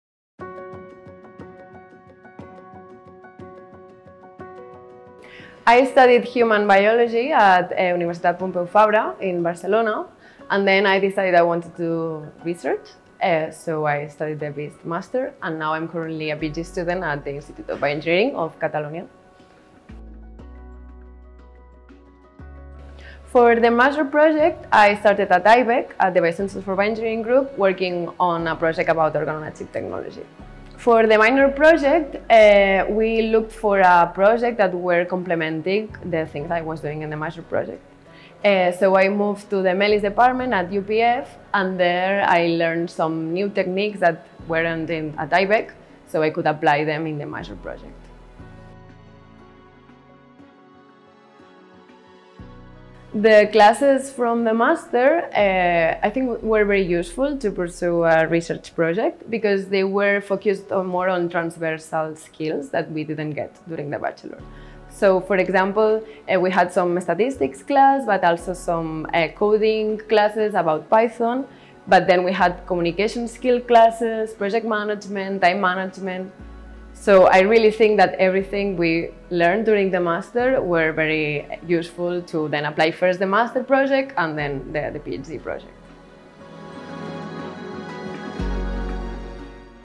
BIST Master Alumni Testimonial - Ainoa Tejedera
Learn about the experience of Ainoa Tejedera in the BIST Master of Research MMRES. Ainoa was a Master Student from the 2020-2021 class. Later on, she enrolled in a PhD at IBEC, one of the BIST Community research centres.